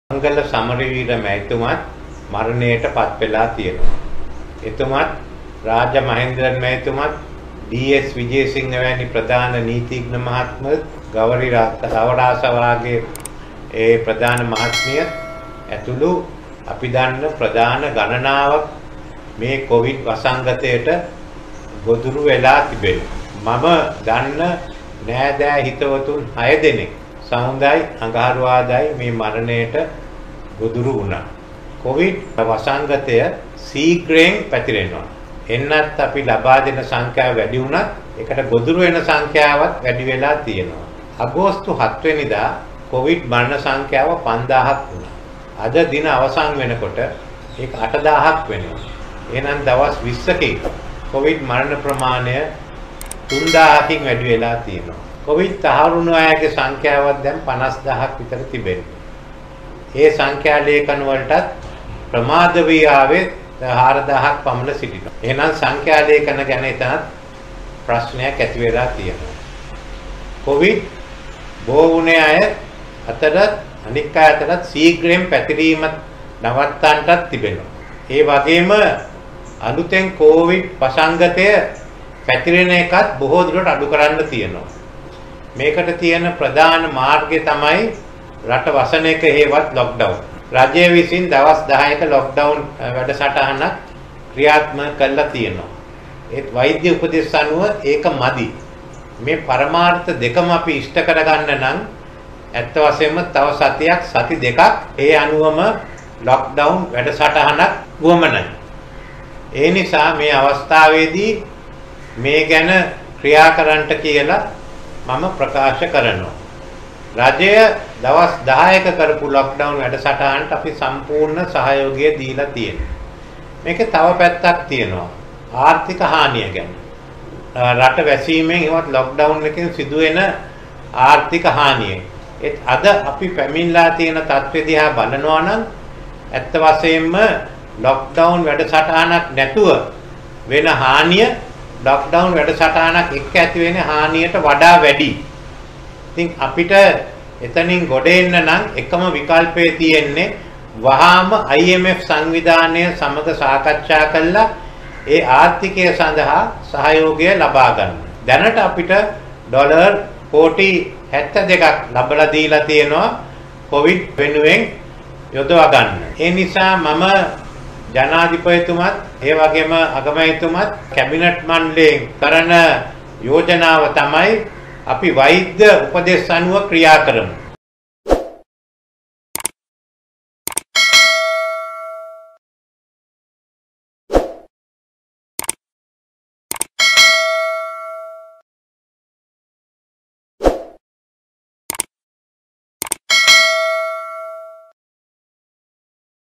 Ranil wickramasinghe speech about covid-19-මංගල, කිලී අපිට නැතිවුණා තවත් ජීවිත නැති කරගන්න බැහැ
තවත් සති දෙකක් රට වහයිද ?.මංගල, කිලී අපිට නැතිවුණා තවත් ජීවිත නැති කරගන්න බැහැ.-IMF එකින් ඩොලර් කොටි 2ක්  දුන්නා/රනිල්ගෙන් විශේෂ ප්‍රකාශයක් du world tv